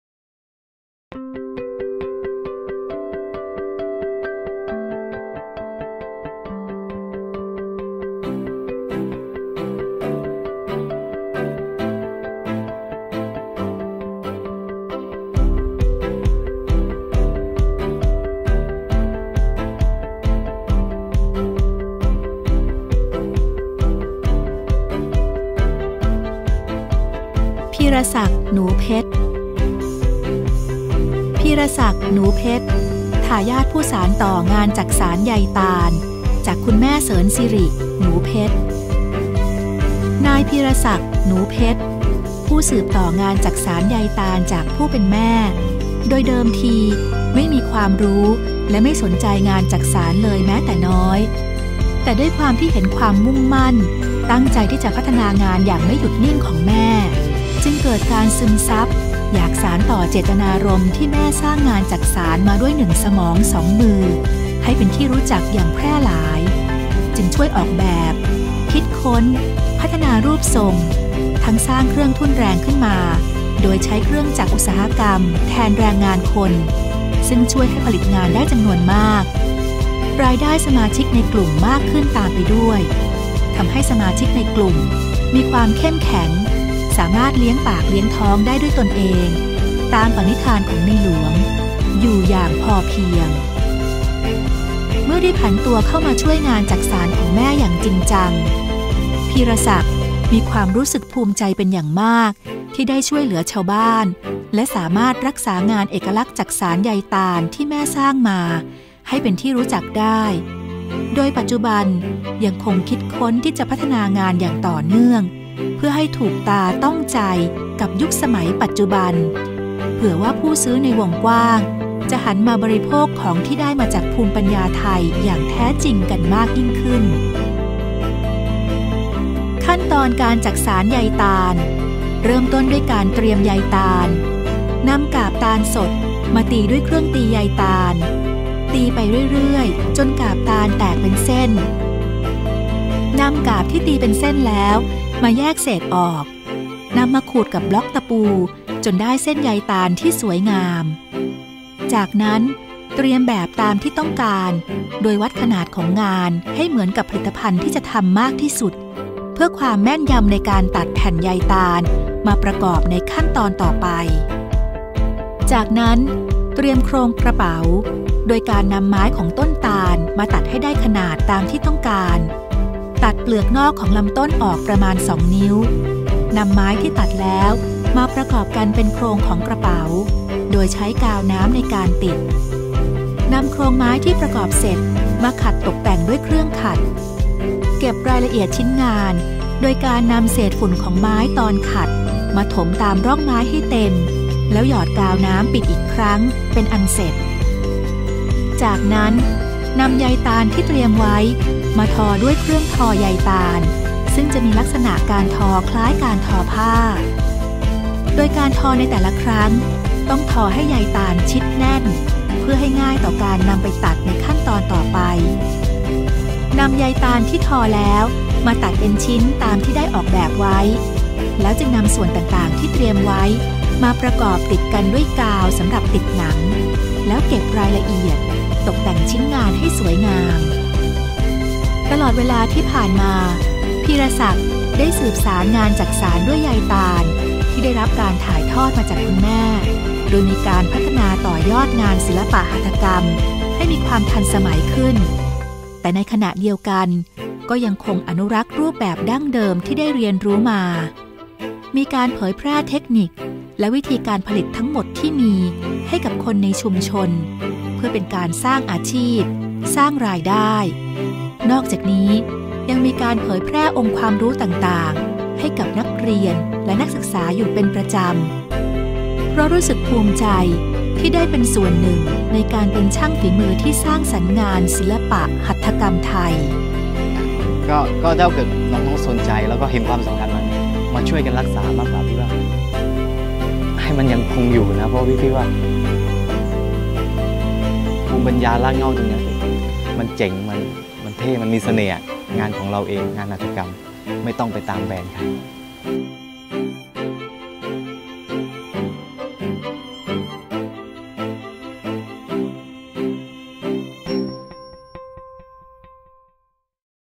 ทายาทช่างศิลปหัตถกรรม 2559 : พีระศักดิ์ หนูเพชร
ทายาทช่างศิลปหัตถกรรม 2559 : พีระศักดิ์ หนูเพชร - ประเภทงานเครื่องจักสาน (ใยตาล) จังหวัดสงขลา